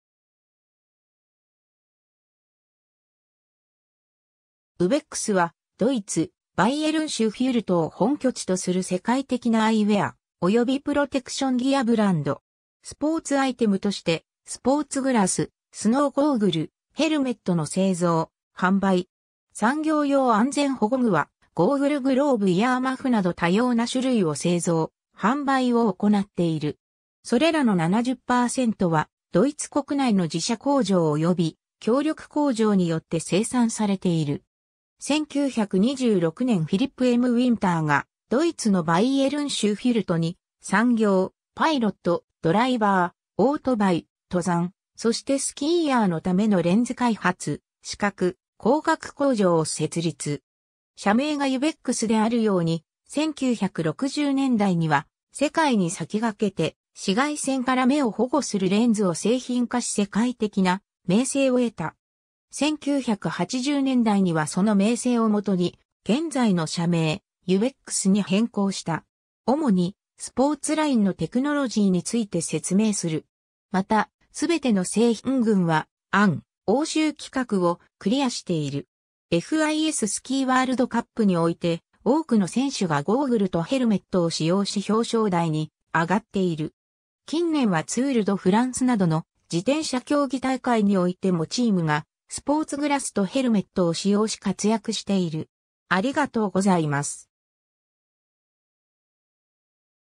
ウベックス
ウベックス（UVEX uvex WINTER HOLDING GmbH & Co.KG）は、ドイツ、バイエルン州フュルトを本拠地とする世界的なアイウェア及びプロテクションギアブランド。
スポーツアイテムとしてスポーツグラス、スノーゴーグル、ヘルメットの製造・販売。
産業用安全保護具はゴーグル・グローブ・イヤーマフなど多様な種類を製造・販売を行っている。
それらの70%はドイツ国内の自社工場及び協力工場によって生産されている。
 1926年フィリップ・M・ウィンターがドイツのバイエルン州フュルトに産業、パイロット、ドライバー、オートバイ、登山、そしてスキーヤーの為のレンズ開発「視覚、光学工場」を設立。
社名がUVEX（UV：ultraviolet[紫外線] EX：exclusded[排除]）であるように、1960年代には世界に先駆けて紫外線から目を保護するレンズを製品化し世界的な名声を得た。
1980年代にはその名声をもとに現在の社名「UVEX（ウベックス）」に変更した。
 主にスポーツラインのテクノロジーについて説明する。
また、全ての製品群（スポーツグラス・ゴーグル・ヘルメット・プロテクター）はEN欧州規格をクリアしている。
 FISスキーワールドカップにおいて多くの選手がゴーグルとヘルメットを使用し表彰台に上がっている。
近年はツール・ド・フランスなどの自転車競技大会においてもチームがスポーツグラスとヘルメットを使用し活躍している。